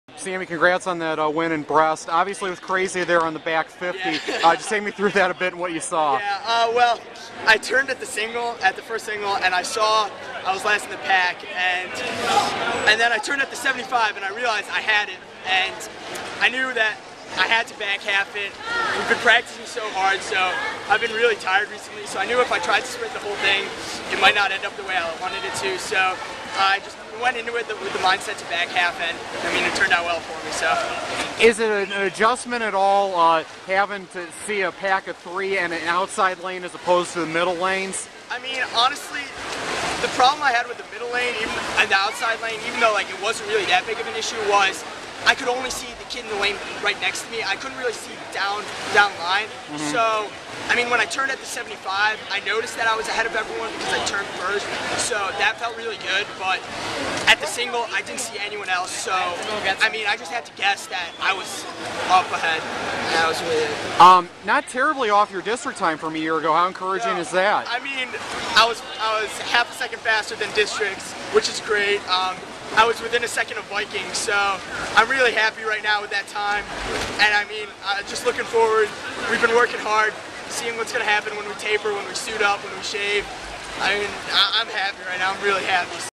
NHswimming @hawkenathletics' @samifares123 discusses winning 100 breast at Orange Invitational
By: Chris Lillstrung - I'm the high school soccer, hockey, swimming and track and field beat writer for The News-Herald.